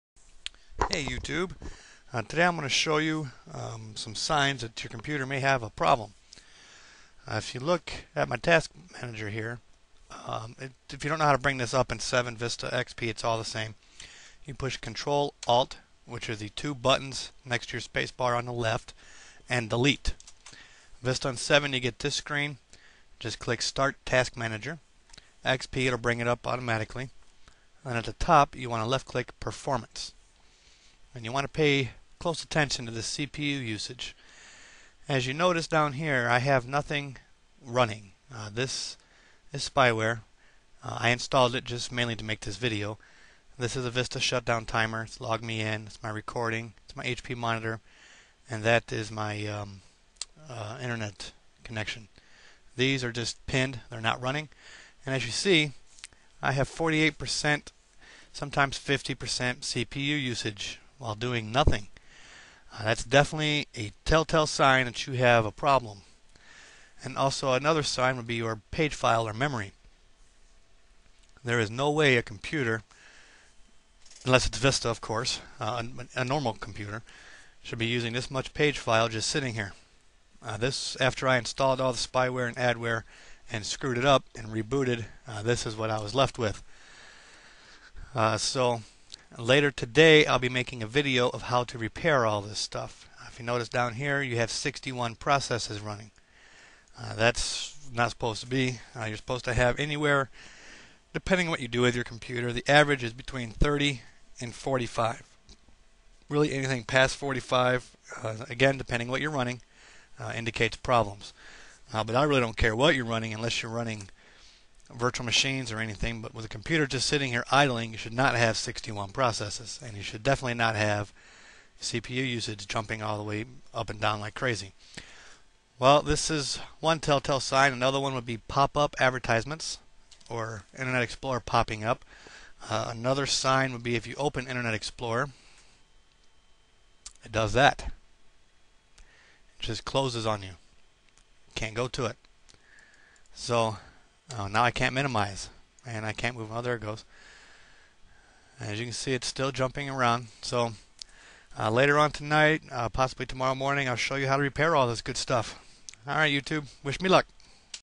signs your infected with spyware or viruses.wmv
I show you signs that you might be infected with spyware, adware,viruses, or some other type of spyware.